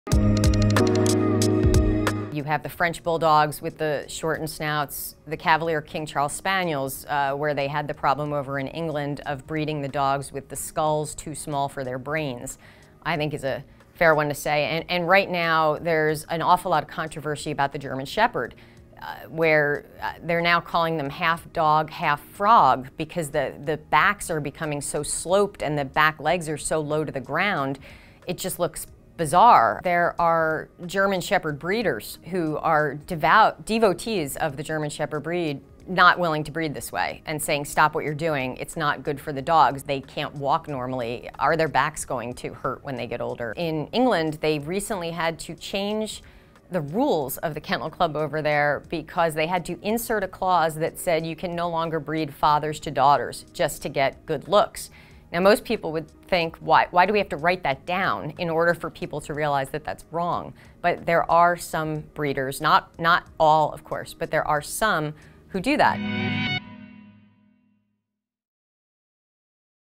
These are the 3 dogs with the most health problems
Kim Kavin is the author of "The Dog Merchants." She explains which dog breeds have the most health problems.